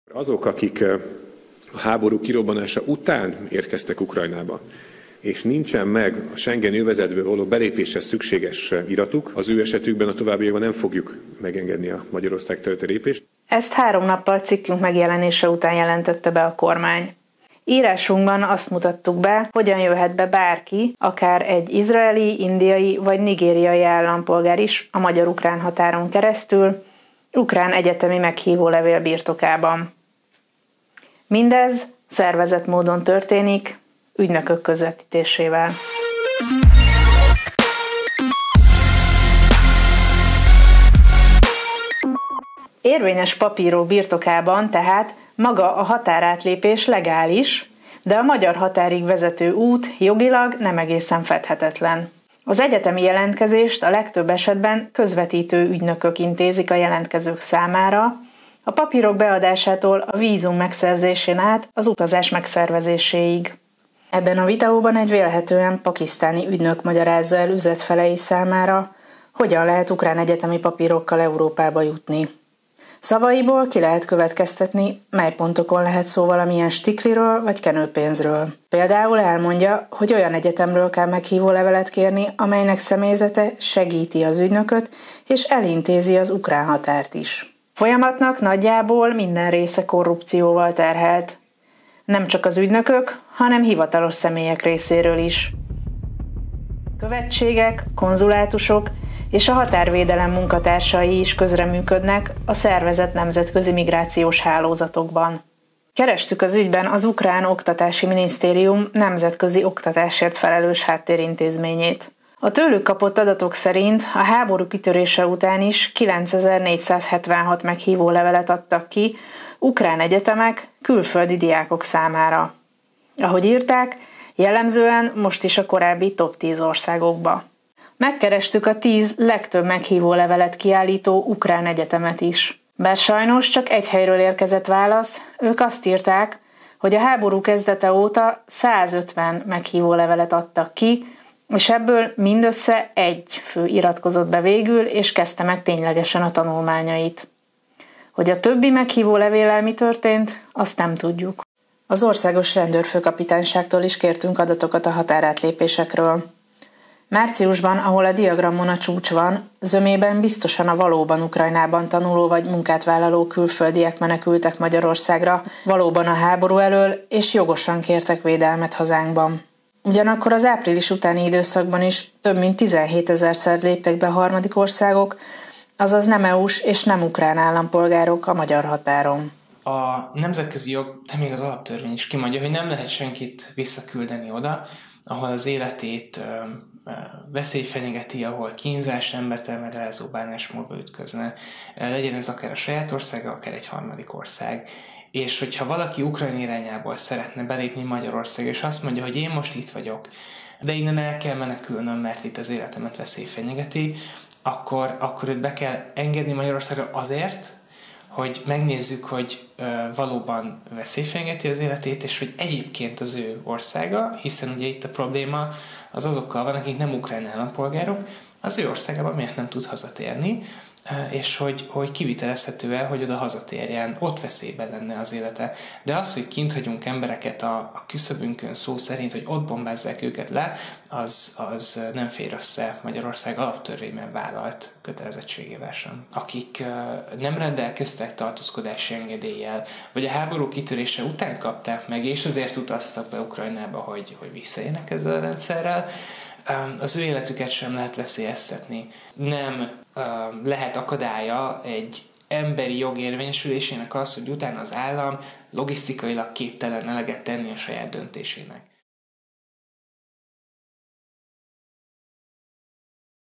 Migrációs kiskapu az ukrán-magyar határon: videón a módszer és a kormányzati válasz
Korábbi cikkünkben részletesen írtunk a szervezett illegális migrációnak egy legális köntösbe bújtatott válfajáról. Ebben feltártuk, hogy ukrán egyetemi meghívólevél birtokában bárki viszonylag egyszerűen be tudott jönni a magyar-ukrán határon, mert a háborús helyzet miatt a magyar rendőrség lényegében mindenkit beengedett. Cikkünk megjelenése után három nappal a kormány bejelentette az ukrán határon történő belépés feltételeinek szigorítását. Jogvédők szerint azonban a szabályozás jogellenes. Összefoglaló anyagunk itt olvasható